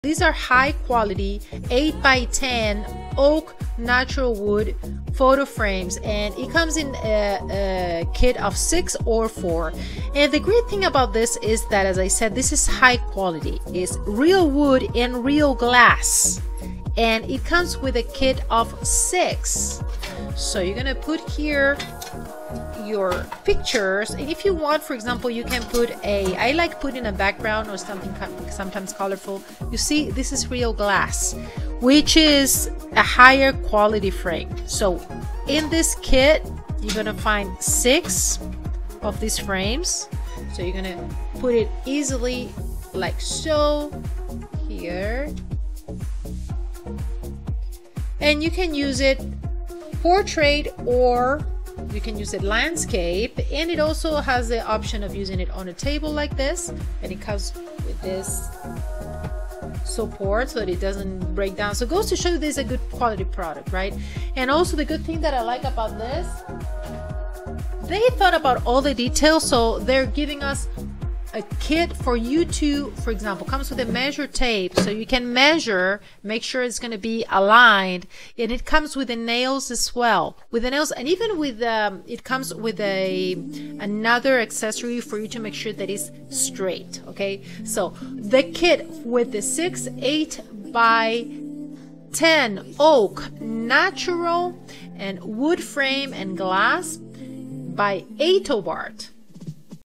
ATOBART 8x10 OAK Natural Wood Photo Frame Review
Find 8x10 OAK Natural Wood Photo Frame by ATOBART on Amazon

8x10 OAK Natural Wood Photo Frame by ATOBART live video review by A-List Amazon Influencer and Singer-songwriter Lorna.

8x10 OAK Natural Wood Photo Frame,6 Pack Picture Frames Real Glass front Wall Hanging Gallery for Wall and Tabletop Display,set of 6